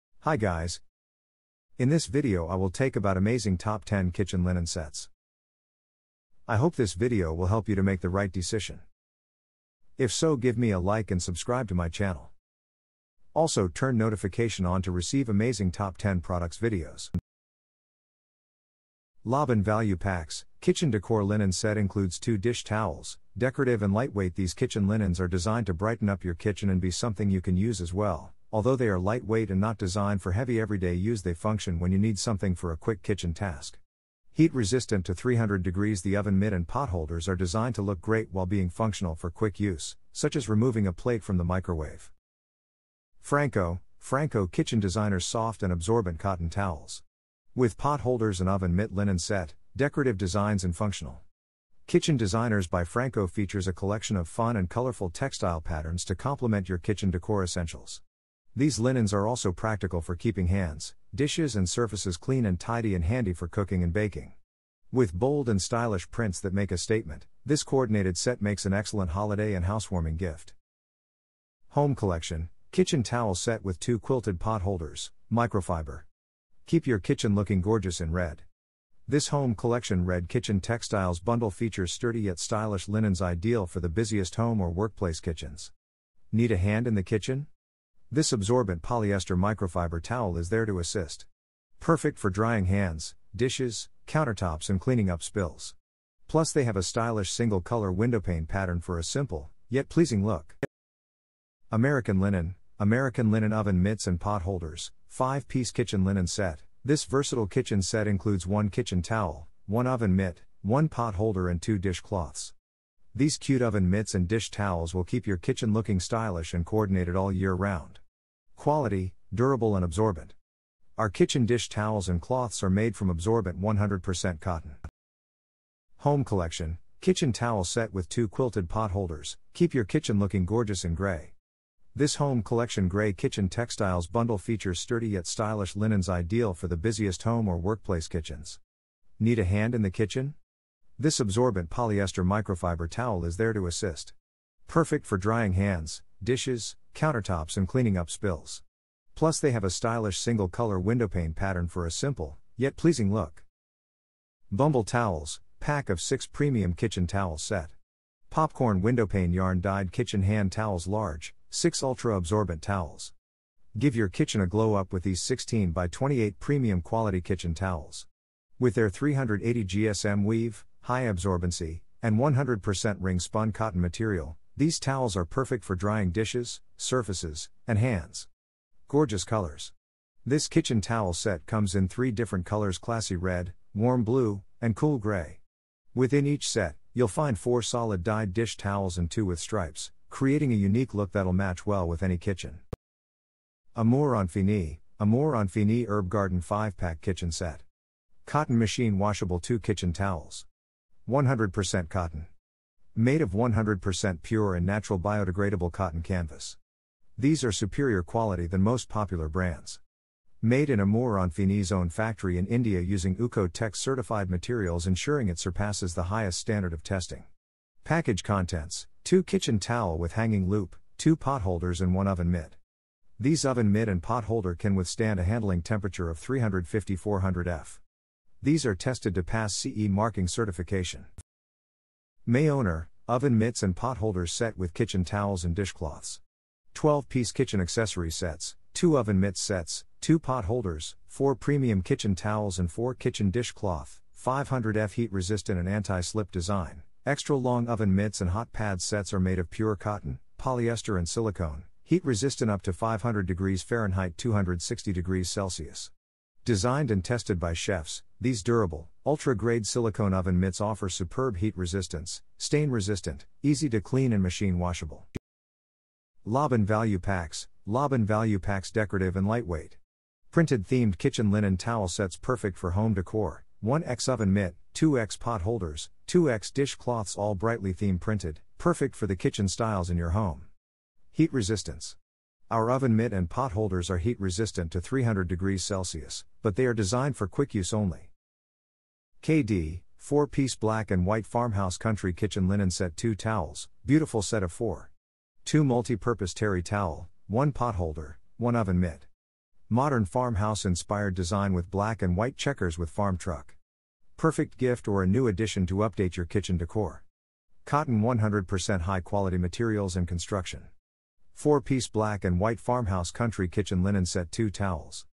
Top 10 Kitchen Linen Sets You Can Buy July 2022
Kitchen Decor Linen Set Includes 2 Dish Towels

Franco Kitchen Designers Soft and Absorbent Cotton Towels with Pot Holders and Oven Mitt Linen Set

American Linen Oven Mitts and Pot Holders - Kitchen Towels and Dish Cloths Sets - Oven Mitts - Tea Towels - Dish Cloths Se

Pack of 6 Premium Kitchen Towels Set - Popcorn Windowpane Yarn Dyed Kitchen Hand Towels - Large

AMOUR INFINI Herb Garden 5 Pack Kitchen Set | Cotton Machine Washable | 2 Kitchen Towels

Oven Mitts and Pot Holders Set with Kitchen Towels and Dishcloths

Lobyn Value Packs Decorative and Lightweight Printed Themed Kitchen Linen Towel Sets Perfect for Home Decor  7 Piece

4 Piece Black and White Farmhouse Country Kitchen Linen Set - 2 Towels

Kitchen Towel Set with 2 Quilted Pot Holders

4 Piece Black and White Farmhouse Country Kitchen Linen Set - 2 Towels